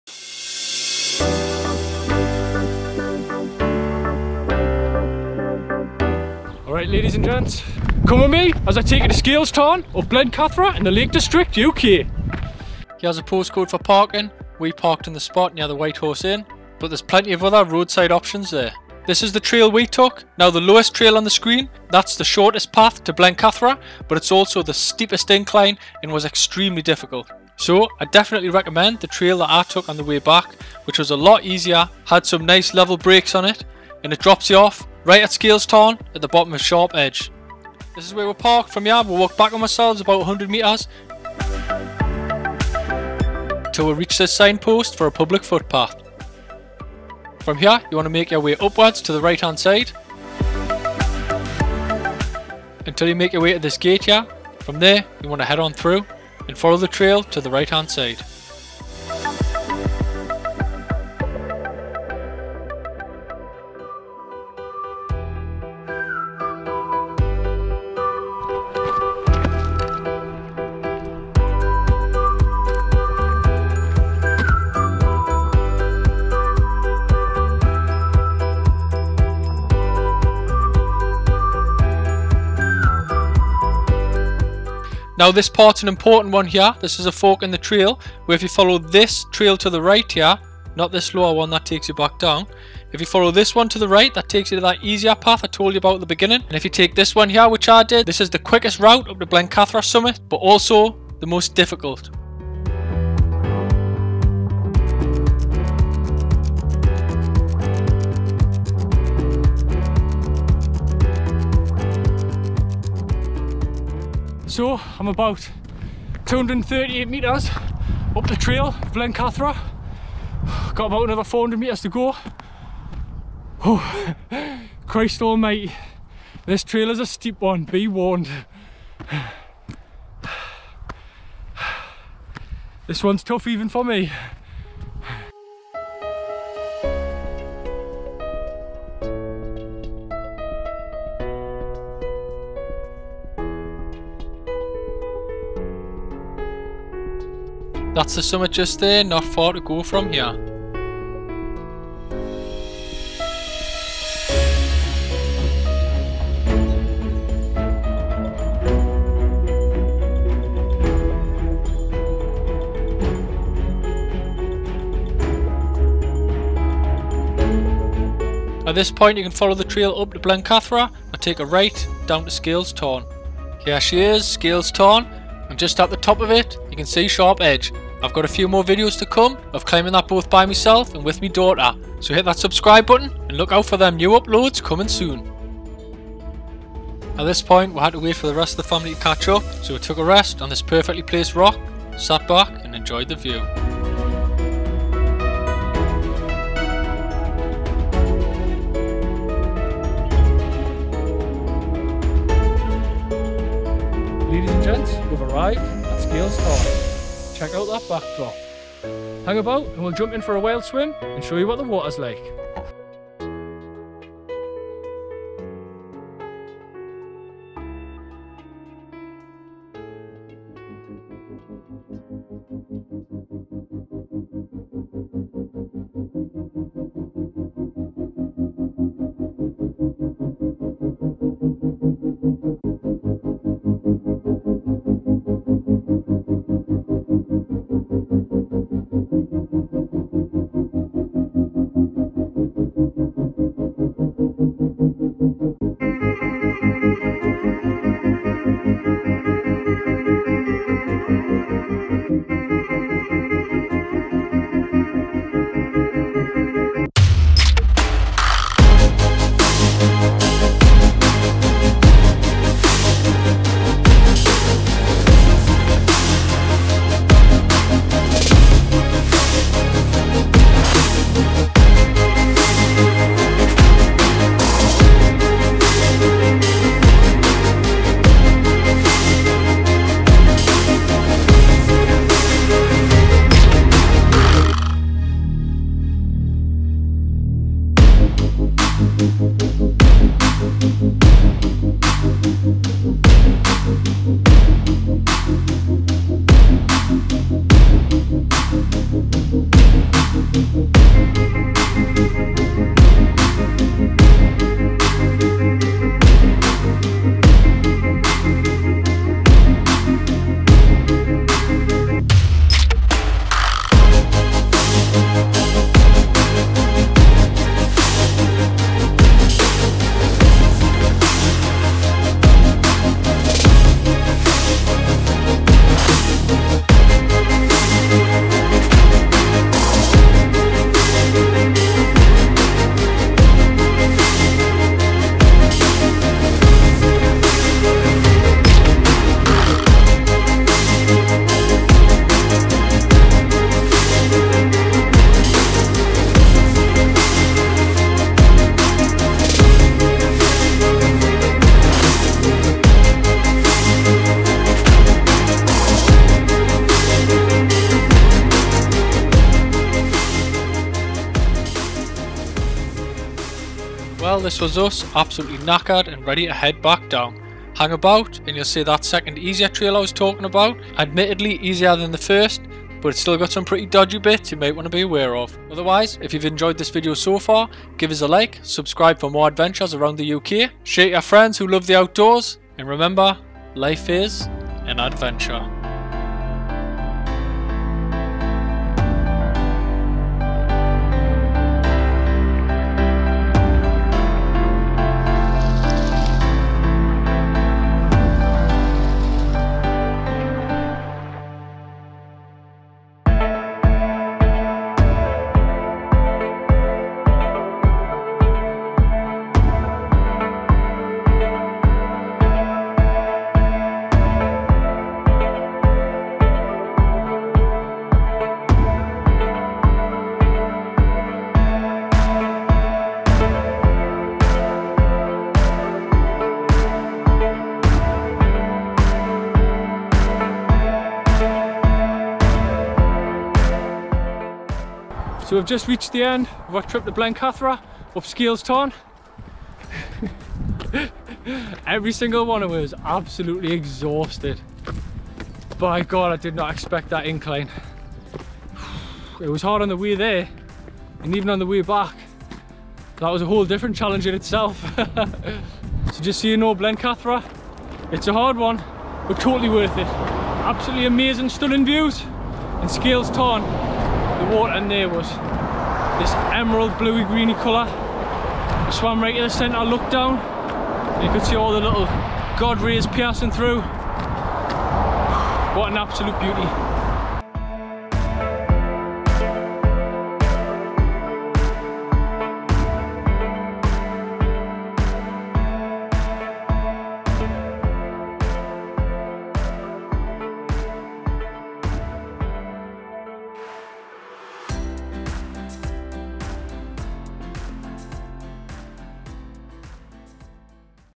Hiking to scales tarn, guide and showcase (Blencathra, lake district uk)
One of my new favorite places in the lake district, a steep hike, a thrilling scramble and a beautiful tarn.
Even better its only 1hr 30mins from the north east where i live rather than the 3 hour journeys i normally make.